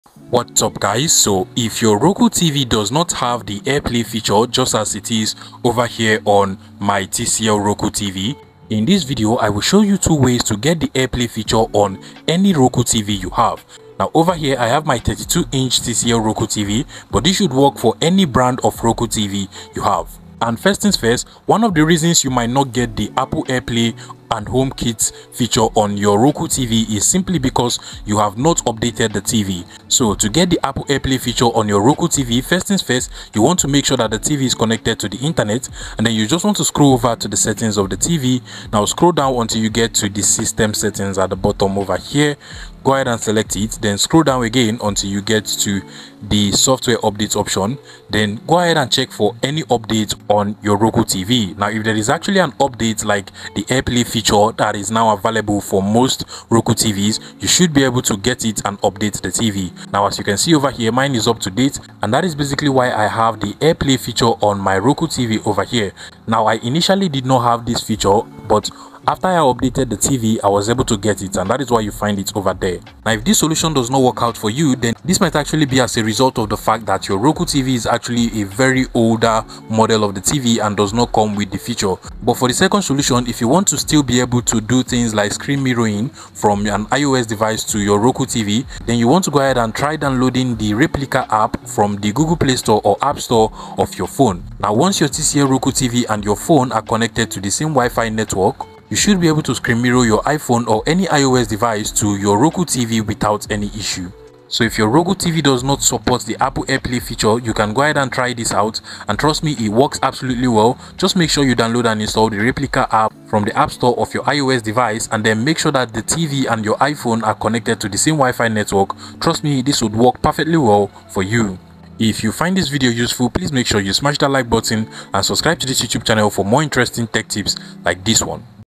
Roku TV Not Showing AirPlay? How to Get Apple AirPlay on Any Roku TV
If your Roku TV doesn't have AirPlay or the feature just isn’t showing, I’ll walk you through exactly how to get Apple AirPlay and HomeKit working on any Roku TV model—including Hisense Roku TV, TCL Roku TV, or Onn Roku TV. In this video, I’ll show you how simply updating your Roku system software can unlock the AirPlay and screen mirroring option. And if your TV still doesn't support it, I’ve got a reliable workaround using the Replica app on your iPhone or iPad that lets you mirror your screen to Roku without AirPlay. Whether you’re trying to stream content, mirror your iOS screen, or connect to HomeKit, this guide will help you get it working on any Roku TV.